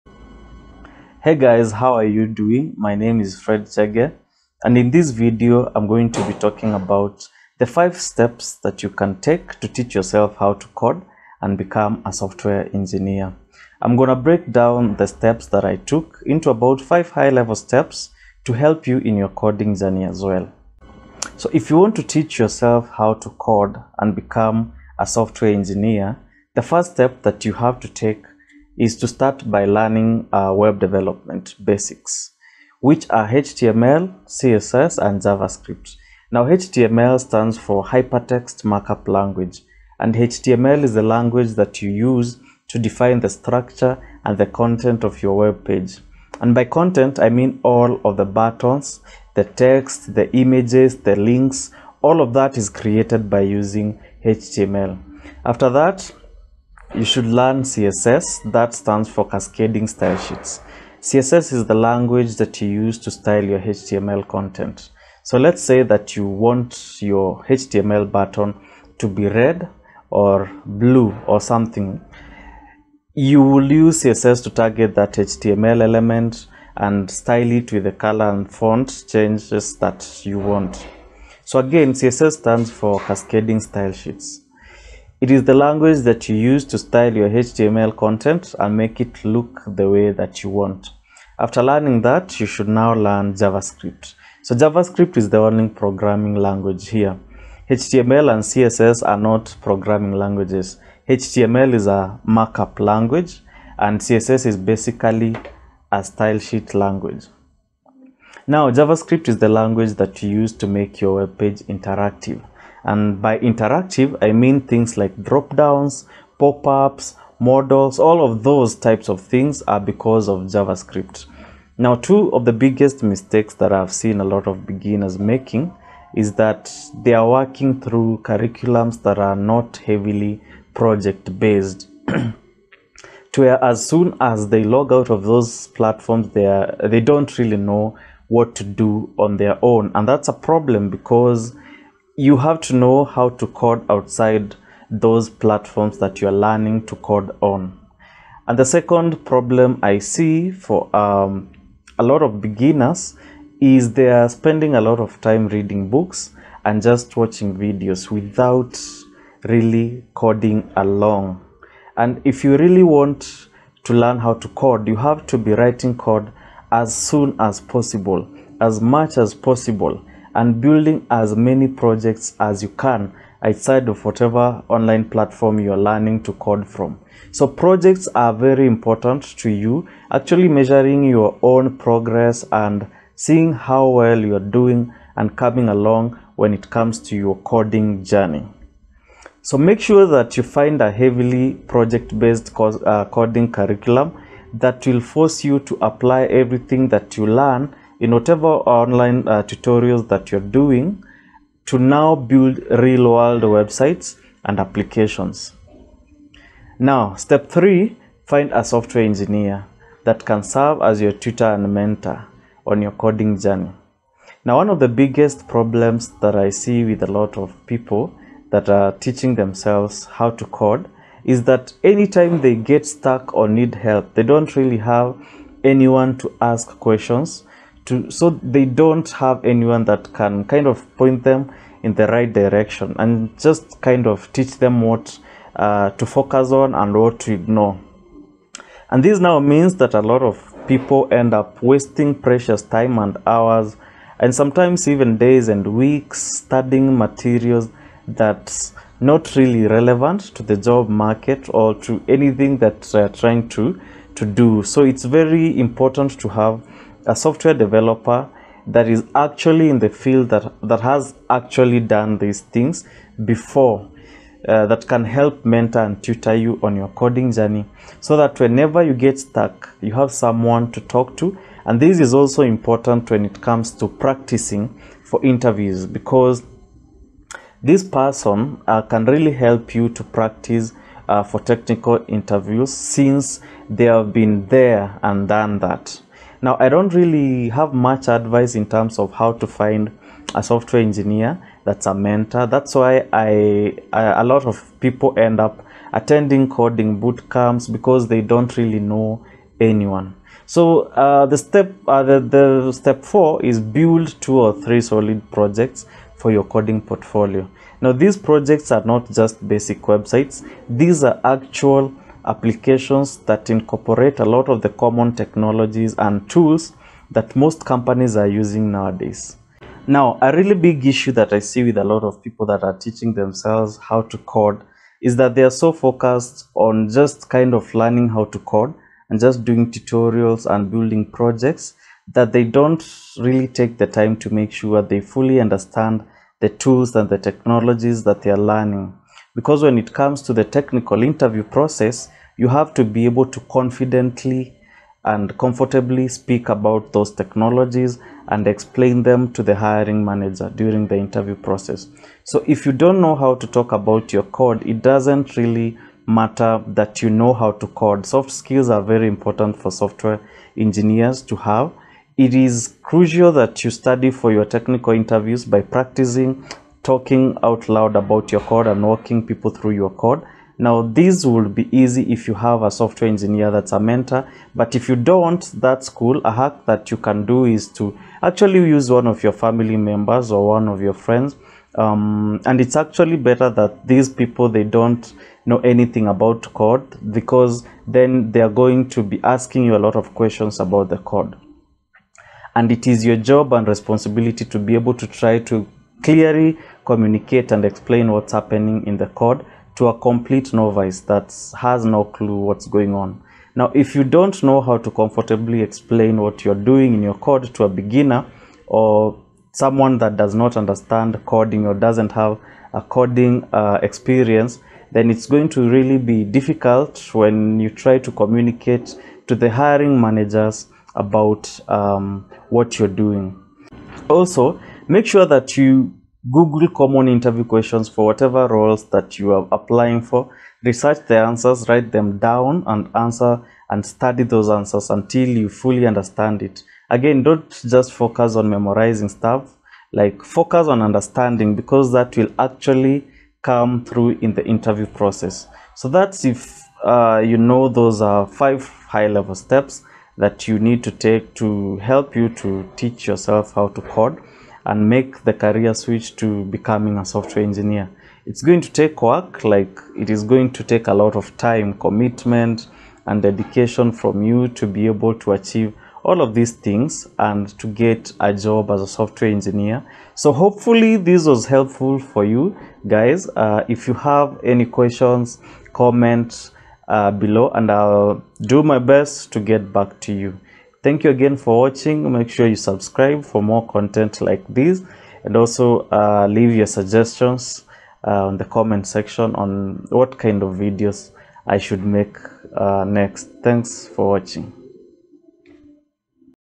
BECOME A SOFTWARE DEVELOPER (NO CS Degree)
Hello! In this video, I'll be telling you steps you should take to kick off your coding journey with the right foundation and preparation to help you make a career switch and become a software engineer. So if you've been considering making a change in your life, and learning to code to become a software engineer is one of the careers you're looking at, this video will walk you through how you can get started and achieve that goal!